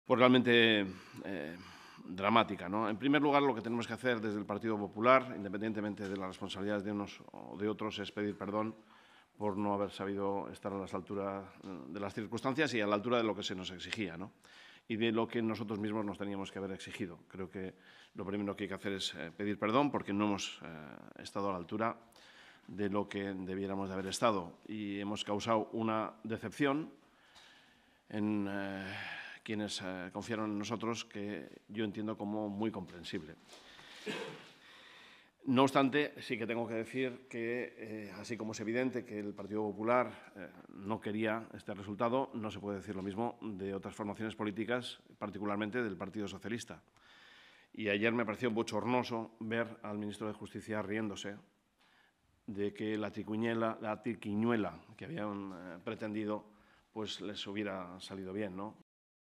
El PP vasco pide "perdón" por no haber estado "a la altura" en la rebaja de penas a etarras
El presidente del PP vasco, Javier de Andrés, ha pedido "perdón" por que su partido a nivel nacional no haya estado "a la altura de lo que se exigía" en la modificación legal en la computación de penas que podría beneficiar a presos de ETA. De hecho, ha reconocido que ve "comprensible" que esto genere "decepción entre quienes confiaron" en su formación al tiempo que ha defendido la necesidad de analizar "cuáles han sido los fallos" que han llevado a una "situación no deseada". Todo ello pese a votar a favor de la reforma.